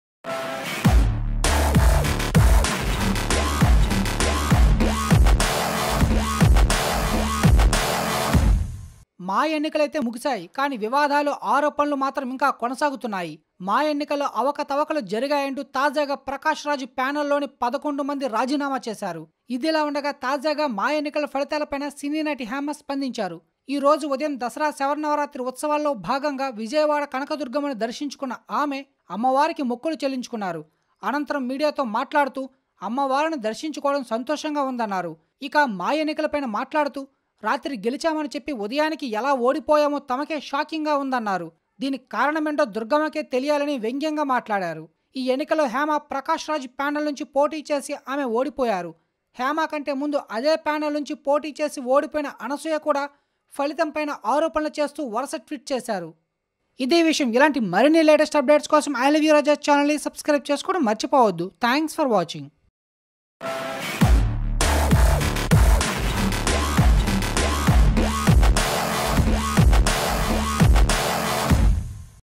Hema Maa Elections | రాత్రి గెలిచి ఉదయాన్నే ఎలా ఓడిపోయారు దుర్గమ్మ కే తెలియాలి : హేమ | I Love U Raja
'మా' ఎన్నికలు అయితే ముగిశాయి కానీ వివాదాలు, ఆరోపణలు మాత్రం ఇంకా కొనసాగుతున్నాయి. మా ఎన్నికల్లో అవకతవకలు జరిగాయంటూ తాజాగా ప్రకాష్ రాజ్ ప్యానల్ లోని పదకొండు మంది రాజీనామా చేశారు. ఇదిలావుండగా తాజాగా మా ఎన్నికల ఫలితాల పైన సినీ నటి హేమ స్పందించింది.

Hema Maa Elections ||  రాత్రి గెలిచి ఉదయాన్నే ఎలా ఓడిపోయారు దుర్గమ్మ కే తెలియాలి : హేమ | I Love U Raja Channel

I Love U Raja Channel